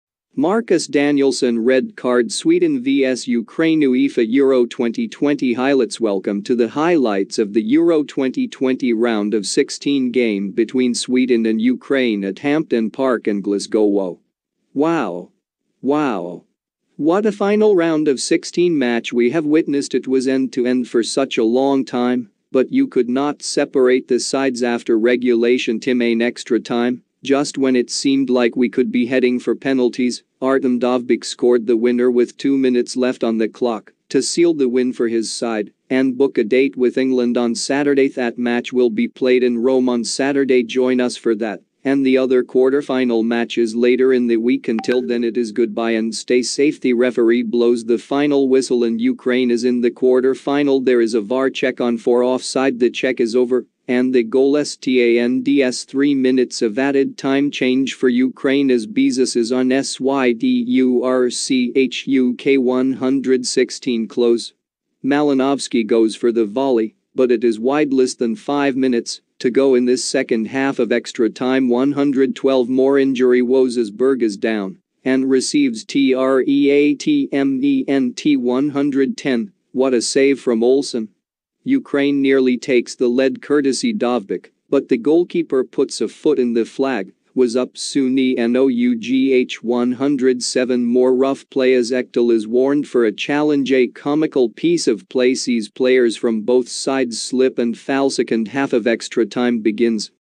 Marcus Danielson Red Card Sweden Vs Ukraine • UEFA EURO 2020 Highlights
Welcome to the highlights of the EURO 2020 Round of 16 game between Sweden and Ukraine at Hampden Park in Glasgow

WOW! WOW! WOW! What a final Round of 16 match we have witnessed It was end to end for such a long time but you could not separate the sides after regulation time

In extra time, just when it seemed like we could be heading for penalties, Artem Dovbyk scored the winner with two minutes left on the clock to seal the win for his side and book a date with England on Saturday

That match will be played in Rome on Saturday Join us for that and the other quarterfinal matches later in the week Until then it is Goodbye and stay safe

The referee blows the final whistle and UKRAINE IS IN THE QUARTER-FINAL!

There is a VAR check on for off side The check is over and the goal stands
3 MINUTES OF ADDED TIME!